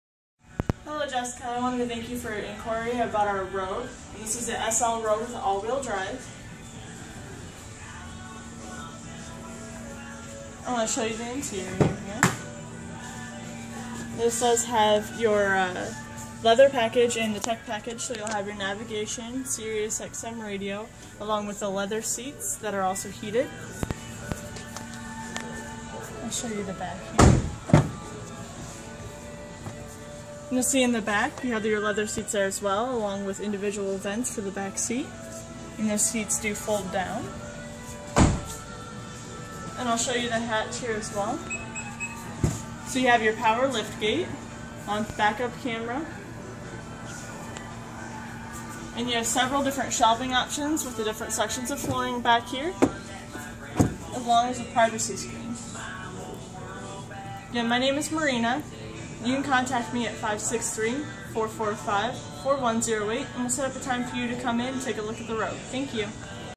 Your New 2015 Nissan Rogue at Lujack Nissan in Davenport Iowa
Mariner Larimer shows you this gorgeous 2015 Nissan Rogue. Located here at Lujack Nissan, 3750 N Harrison Street, Davenport IA, 52806.